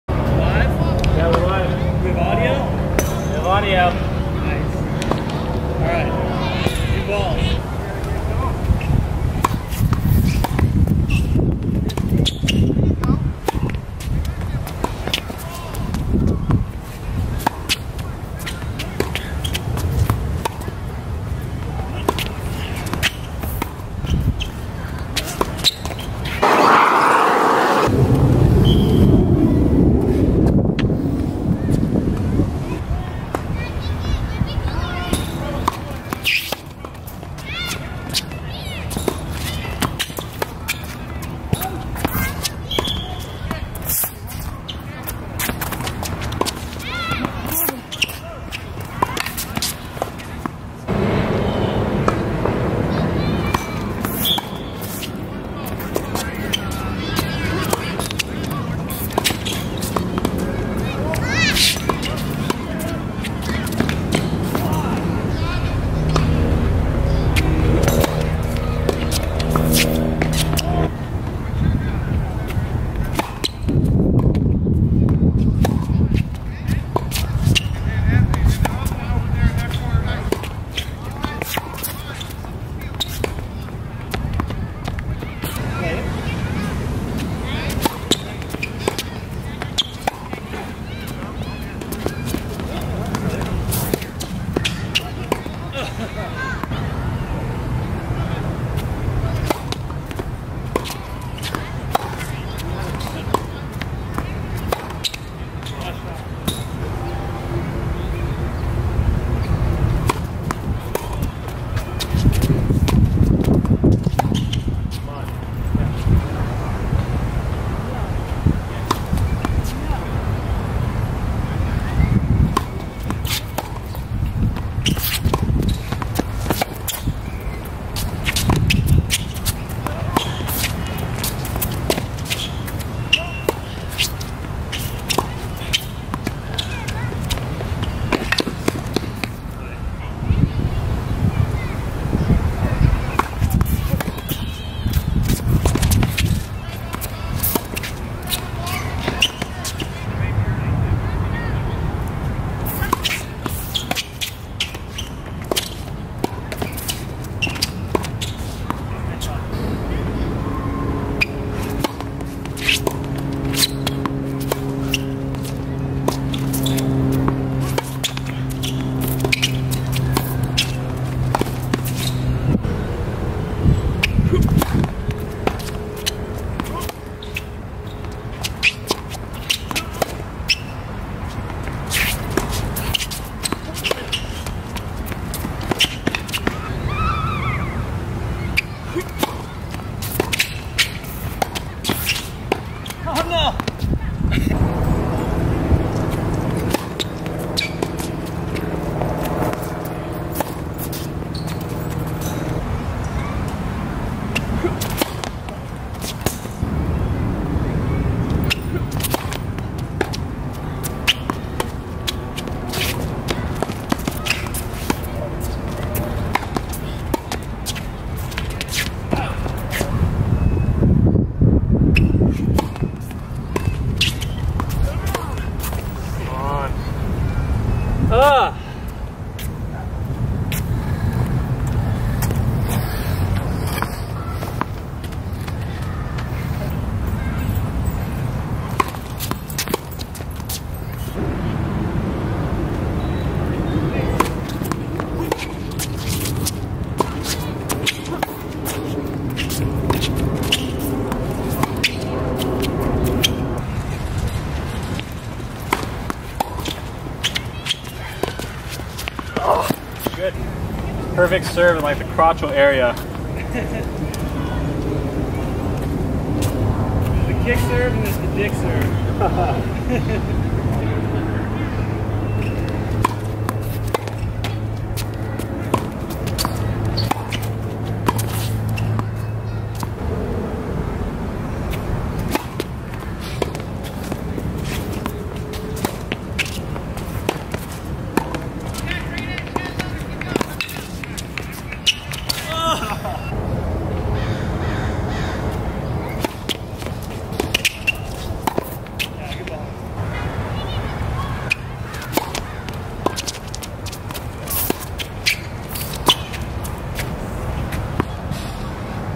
Tennis with Jeff F. - USTA 4.5 SoCal Highlights HD
First tennis session in LA! Warm weather is good for my game :)

Two sets with my buddy Jeff F. who also moved down from SF a little while ago. He plays a high pressure serve and volley game that makes you uncomfortable, but I caught him on an off day and got him 6-1, 6-0 (much closer than the score indicated).

- Camera: Sony a6000 mirrorless
- Tripod: MeFoto Roadtrip Classic (carbon fiber)